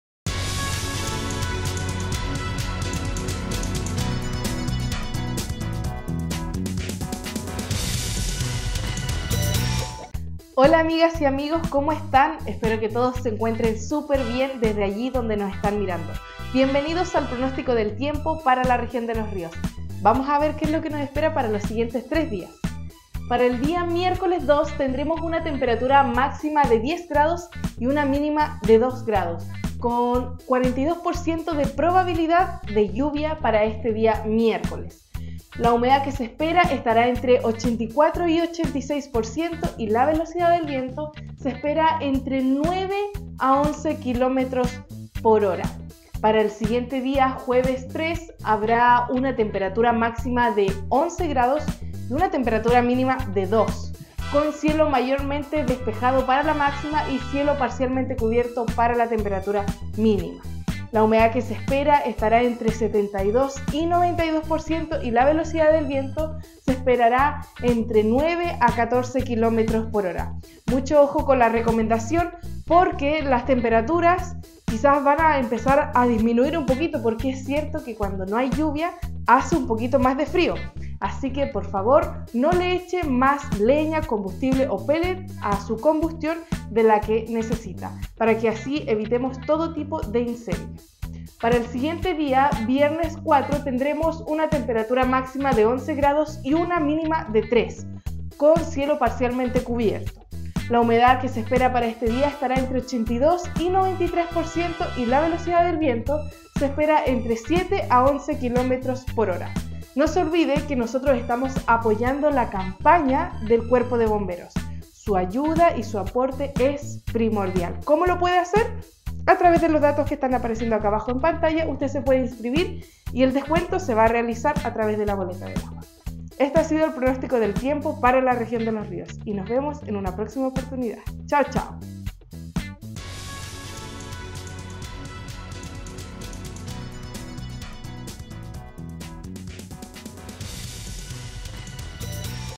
Pronóstico del tiempo del miércoles 2 al viernes 4 de agosto de 2023
Pronóstico del tiempo del miércoles 2 al viernes 4 de agosto de 2023 para Los Ríos